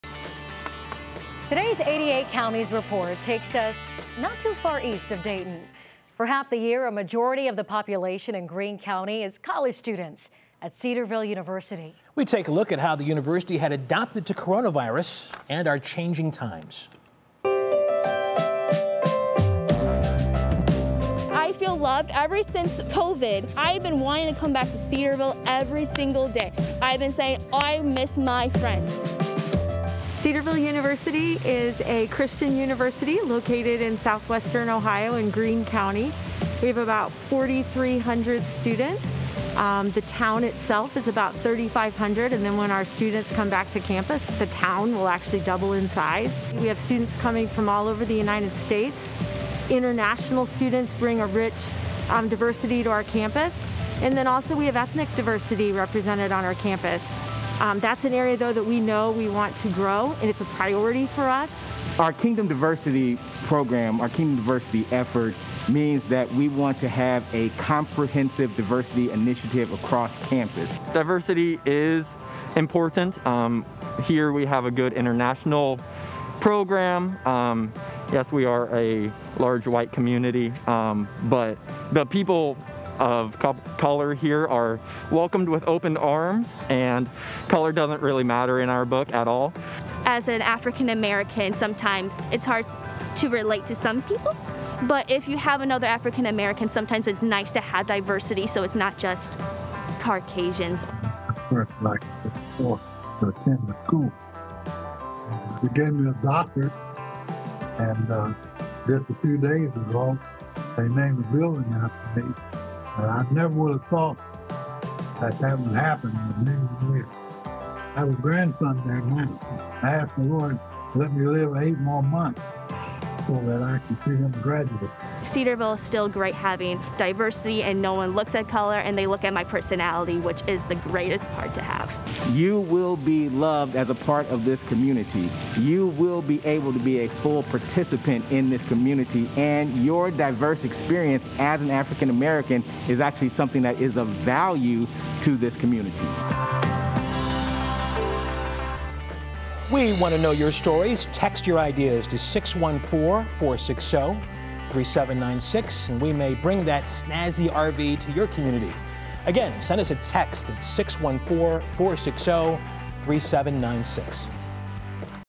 88 counties in 88 days: Greene County college students talk racial justice and COVID-19
3News talked with Cedarville University students about COVID-19